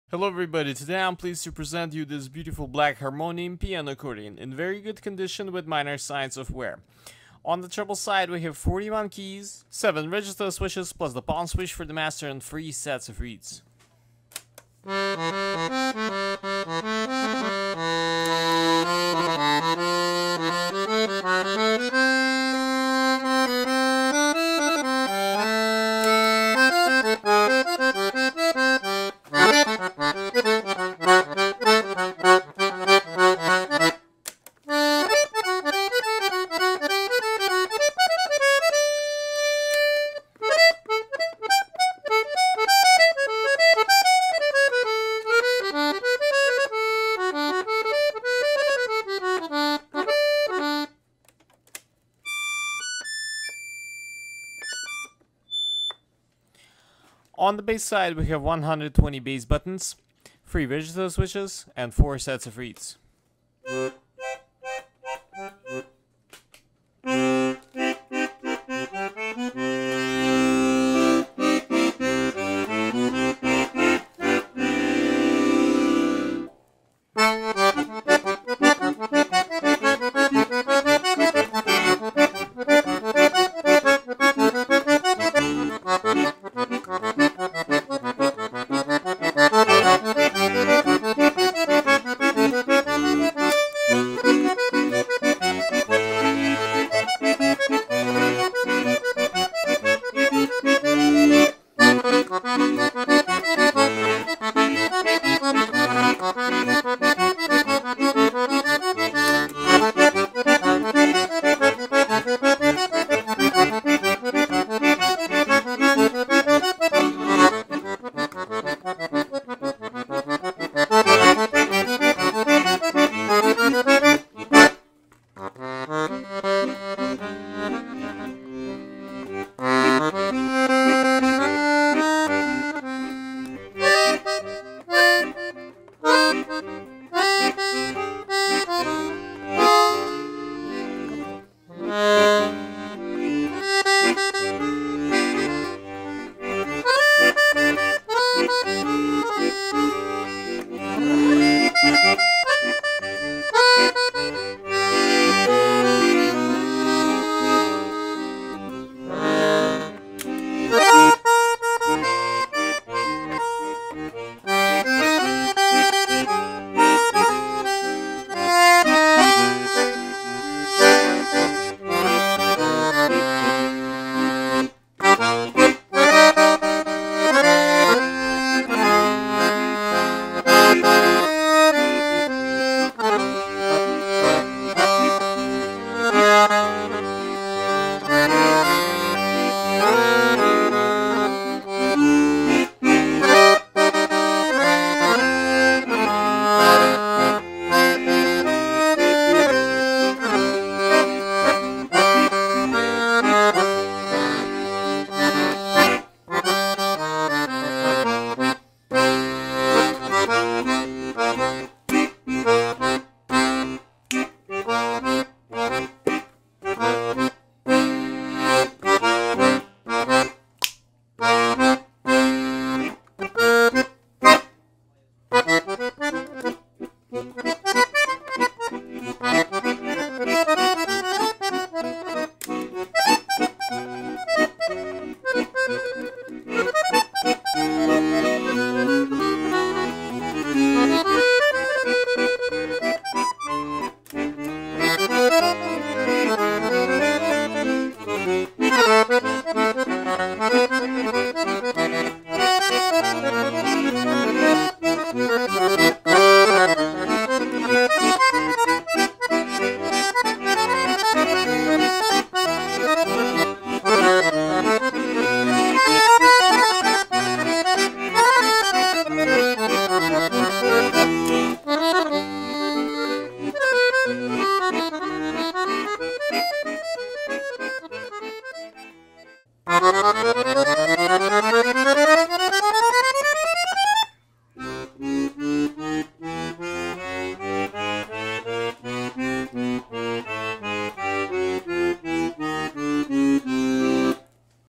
5330 - Black Harmonium De Luxe Hand Made Piano Accordion LMH 41 120 $1199
Brand: Harmonium
Model: Harmonium
Type: Piano Accordion
Reeds: 3/4 LMH
Treble: 41 Keys, 19.25"(cm) Key to Key, 7+M Registers
Bass: 120 Bass Buttons, 3 Registers
Weight: 22lbs / 10kg
Features: Double octave tuning with classic design
Includes: Used Straps and Case, 6 Month Store Warranty